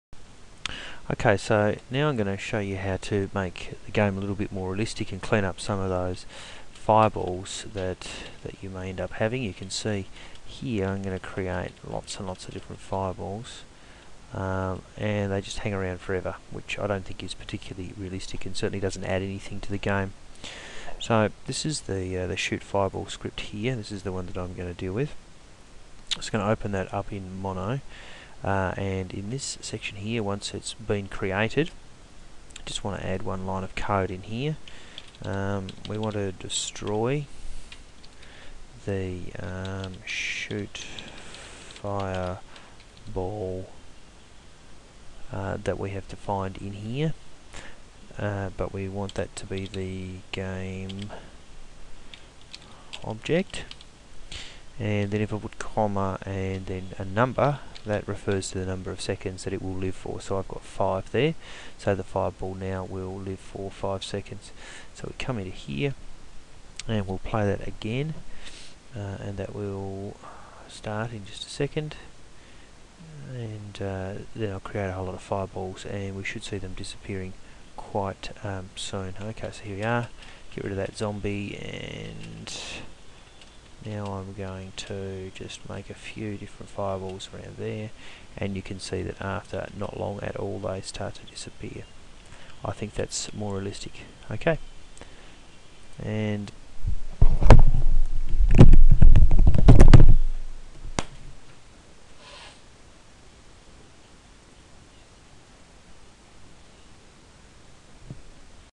Unity 3D part13, Destroy the fireball after a period of time
This will show you how to destroy the fireball after a period of time.  An easy one!